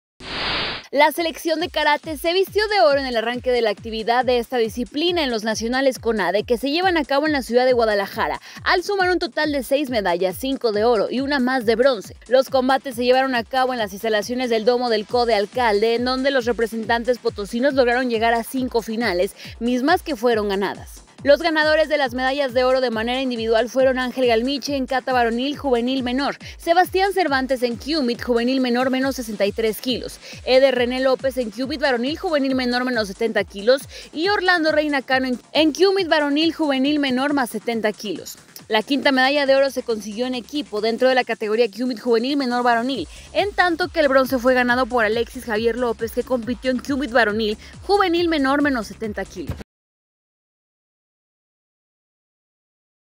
Para San Luis Potosí cinco oros y un bronce en Karate de Nacionales CONADE
SLP.- La selección de karate se vistió de oro en el arranque de la actividad de esta disciplina en los Nacionales CONADE que se llevan a cabo en la ciudad de Guadalajara, al sumar un total de seis medallas, cinco de oro y una más de bronce.

Los combates se llevaron a cabo en las instalaciones del Domo del Code Alcalde, en donde los representantes potosinos lograron llegar a cinco finales, mismas que fueron ganadas.

Los ganadores de las medallas de oro de manera individual fueron Ángel Galmiche en Kata varonil juvenil menor, Sebastián Cervantes en kumite juvenil menor -63 kg., Eder René López en Kumite varonil juvenil menor -70k y Orlando Reyna Cano en Kumite varonil juvenil menor +70kg.

La quinta medalla de oro se consiguió en equipo, dentro de la categoría Kumite juvenil menor varonil; En tanto que el bronce fue ganado por Alexis Javier López, que compitió en Kumite varonil juvenil menor -70kg.